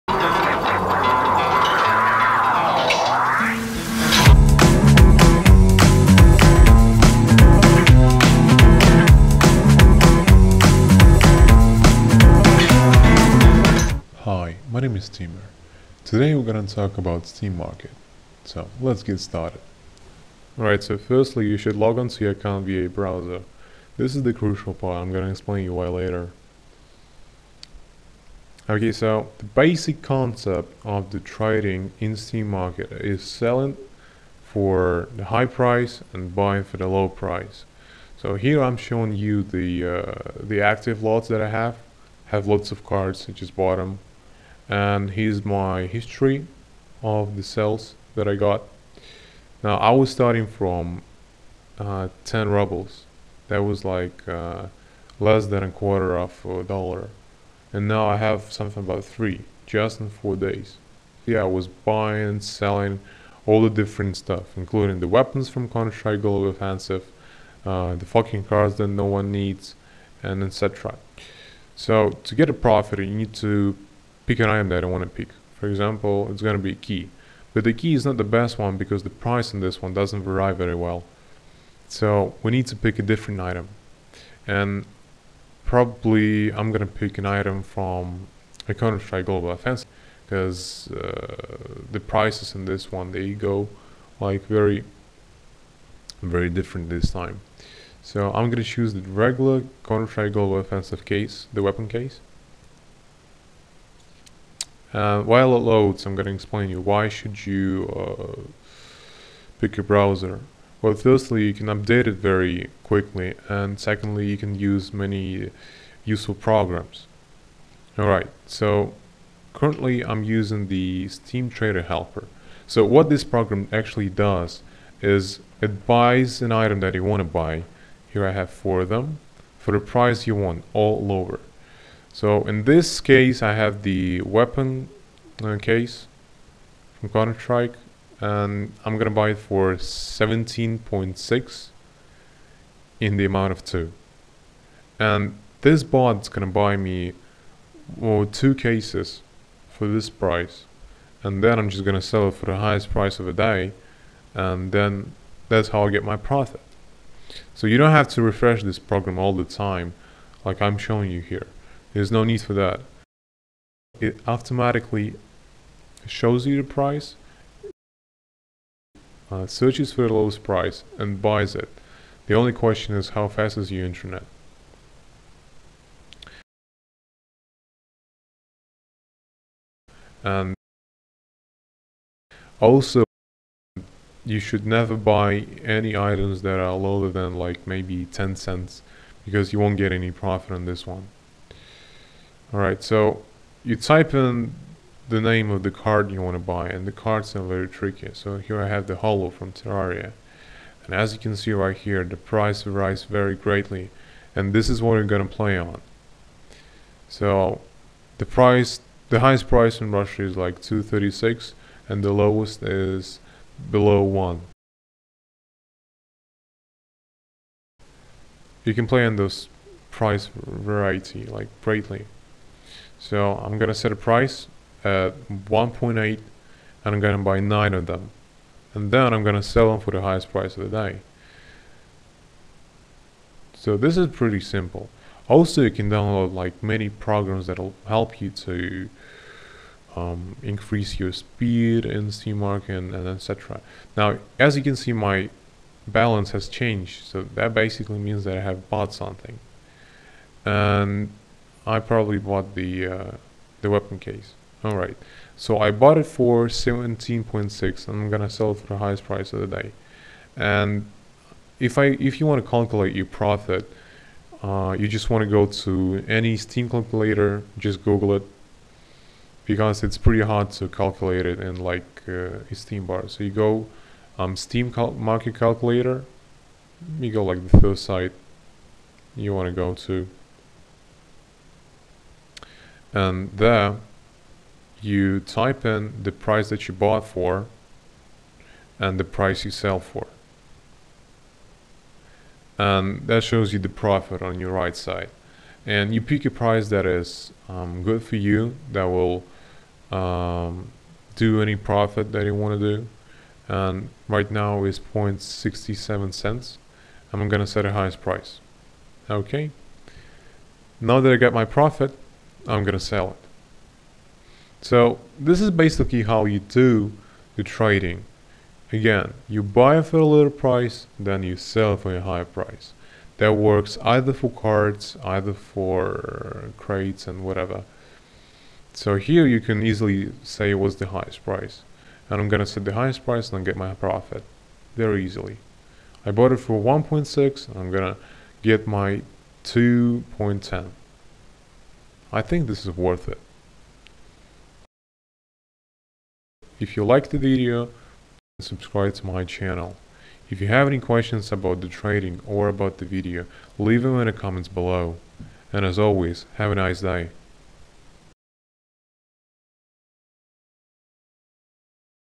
How to get Easy Money (Profit) from Steam Community Market (ENG)
UPDATE : Steam market now uses new system for popular lots, watch the new video explaining how it works

This is a little tutorial about getting profit in Steam Market.
Sorry about little sound lags Vagas encrypted some errors ;D.
If you got any questions feell free to ask me in comments, I'll do my best)